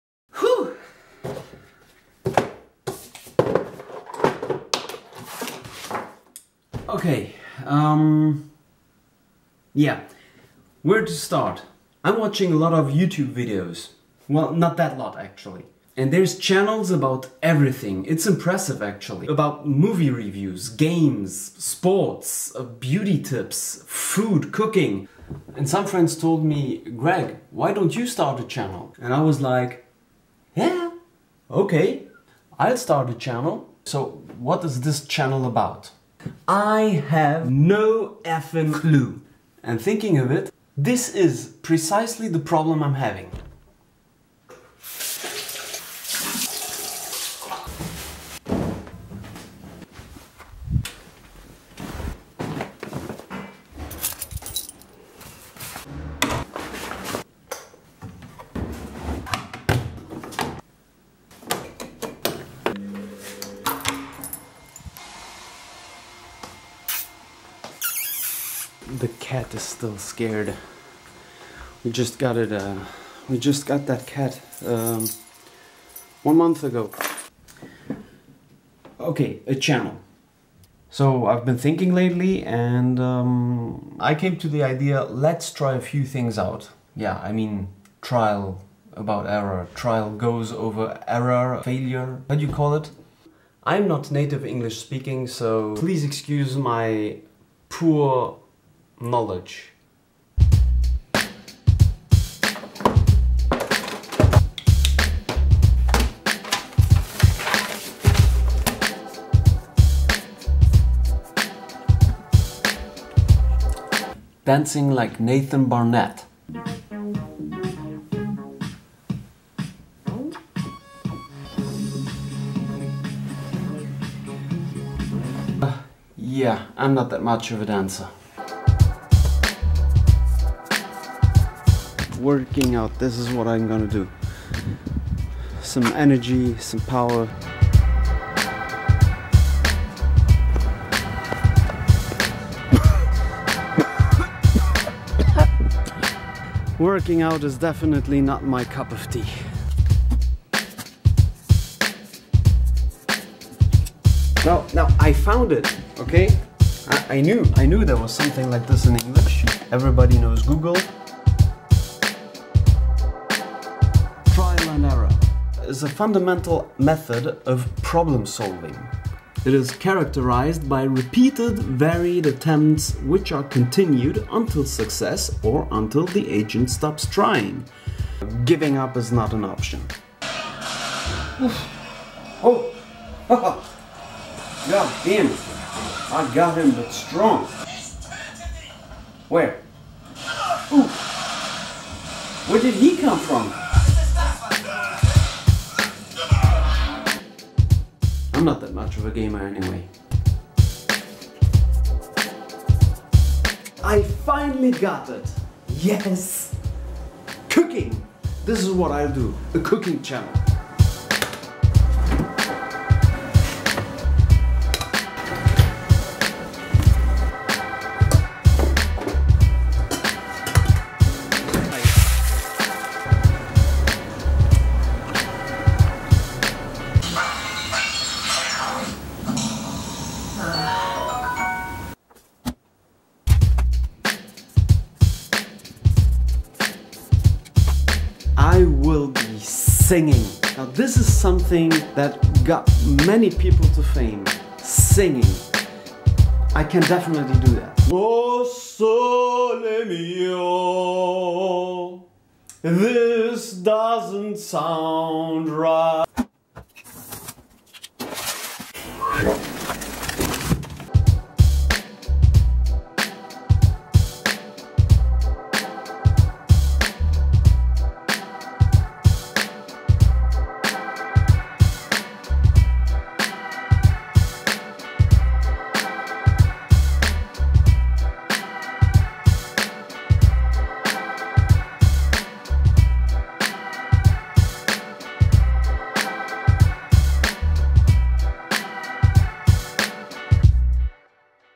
Creating my Channel
This video is the bginning of my channel. Discover the hurdles I go through to actually create my first video for this channel.